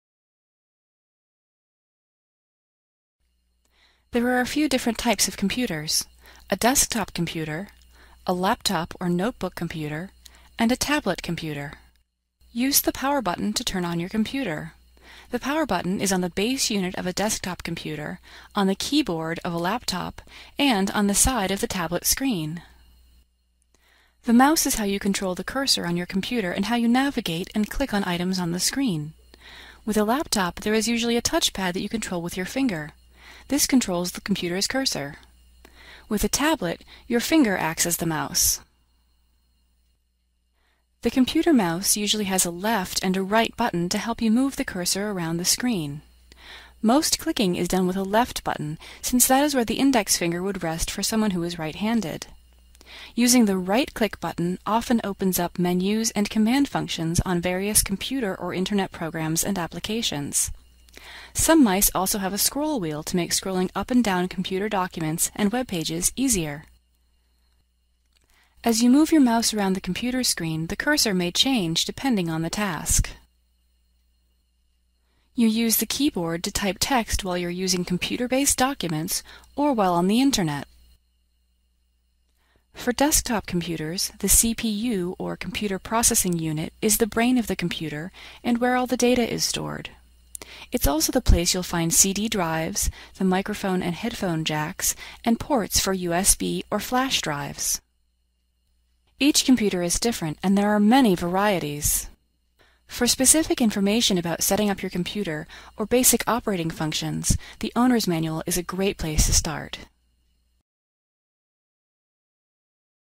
Parts of a Computer
Learn about the different parts of a computer, like the mouse, CPU and start button.